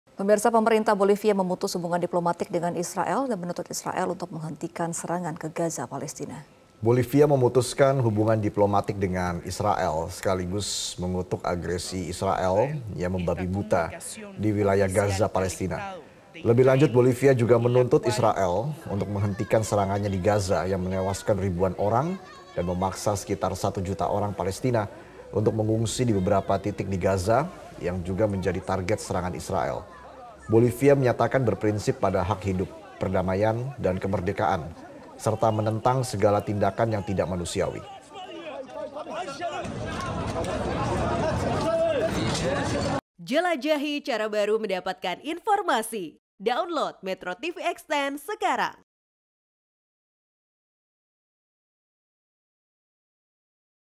Bolivia Putuskan Hubungan dengan Israel
MetroTV, Pemerintah Bolivia memutus hubungan diplomatik dengan Israel dan menuntut Israel untuk menghentikan serangan ke Gaza Palestina.